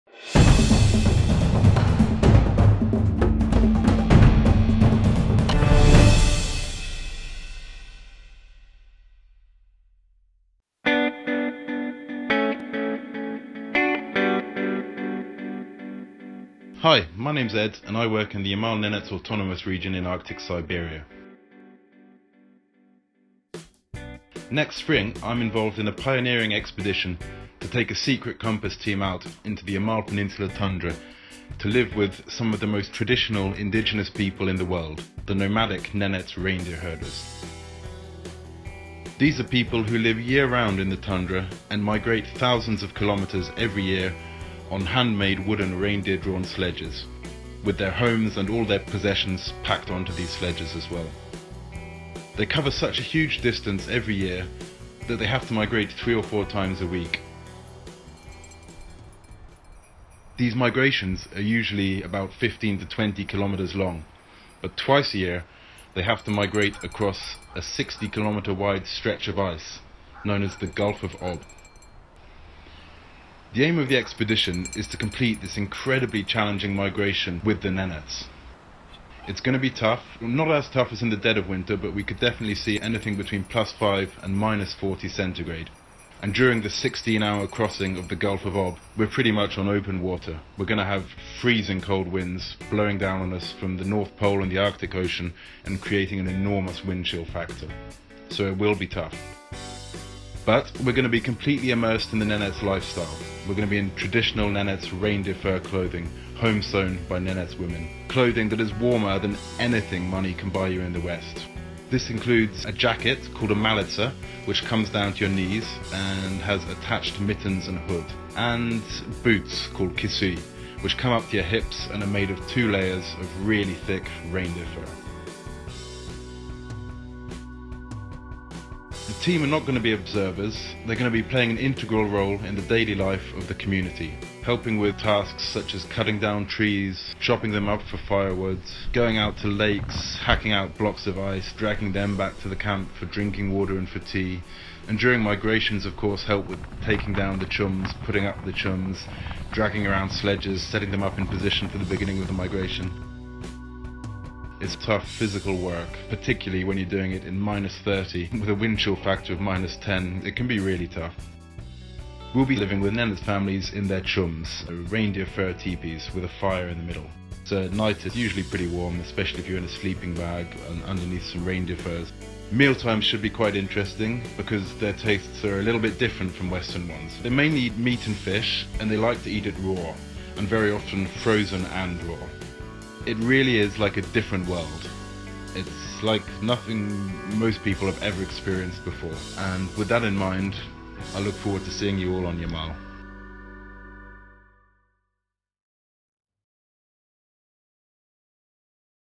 Siberia: A reindeer migration with the Nenets
Journey to the end of the world with the nomadic Nenets reindeer herders as they embark on an epic migration across the frozen Gulf of Ob, deep in the Arctic Circle. Immerse yourself in the Nenets' traditional way of life on this minimalist expedition and join their celebrations at the annual reindeer herding festival.